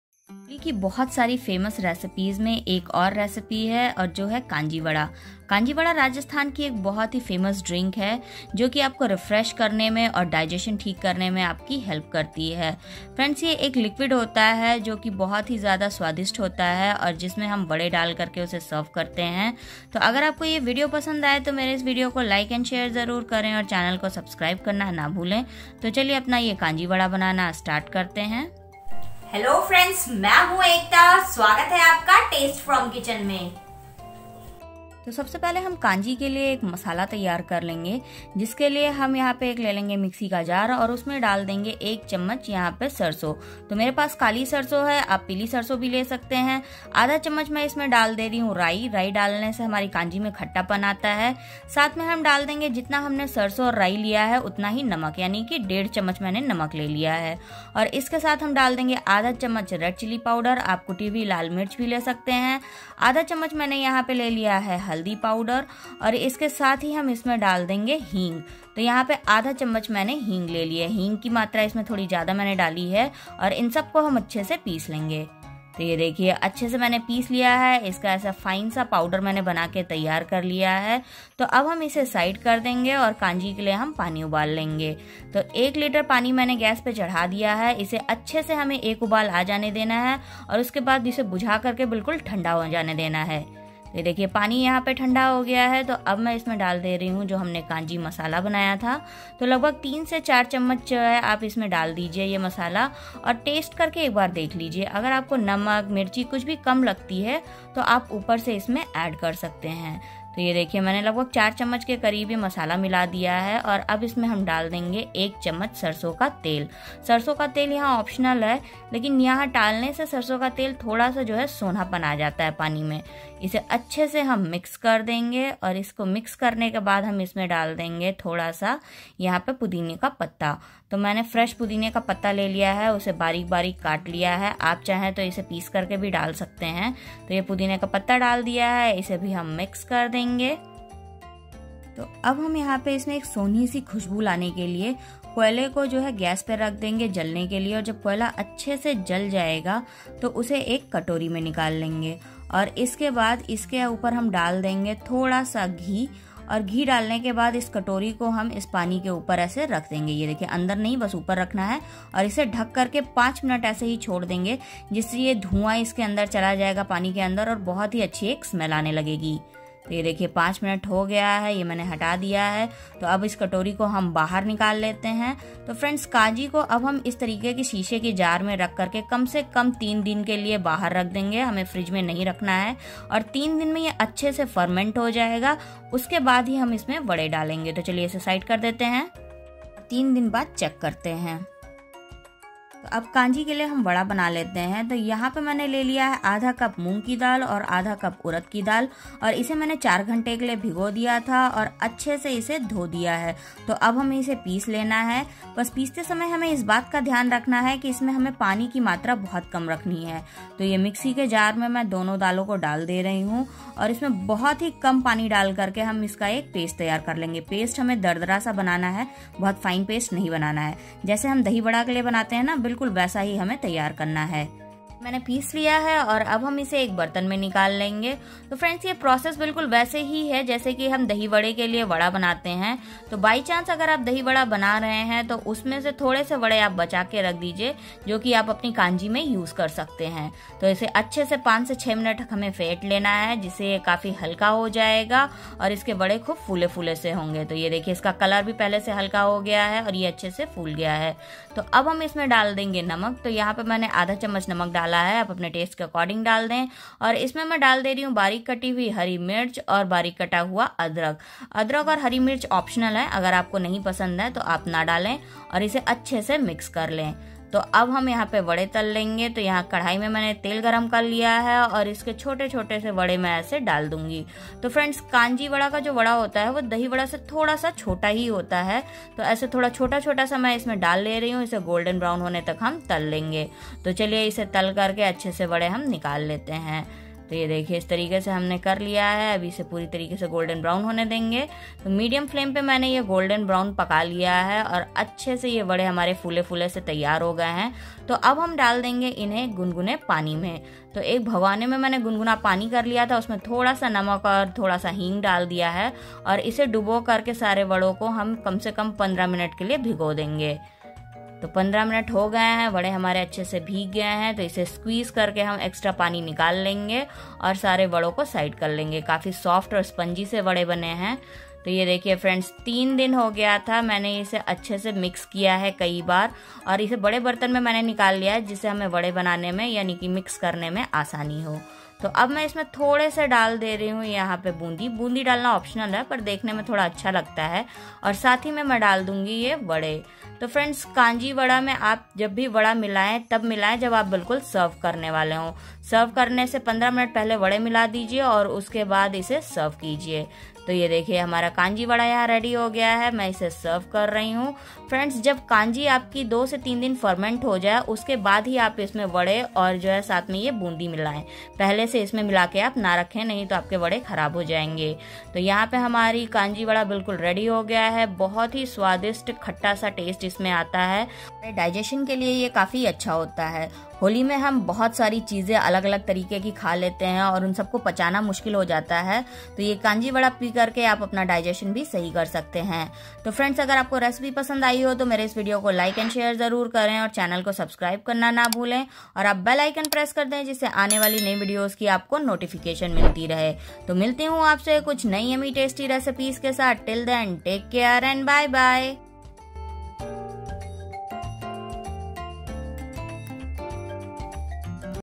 Kanji Vada Recipe | ठेले वाले भैया की सीक्रेट रेसिपी बनाएं घर पर | होली स्पेशल कांजी वड़ा रेसिपी
Ingredients for Kanji Vada
moong dal 1/2 Cup
urad dal 1/2Cup
chopped green chilli 1
chopped Ginger 1/2 tsp
Rai 1/2tsp
hing 1/2tsp
mint leaves
Bundi 1/2cup

OTHER SOCIAL MEDIA ACCOUNT LINKS:-

OTHER RECIPES -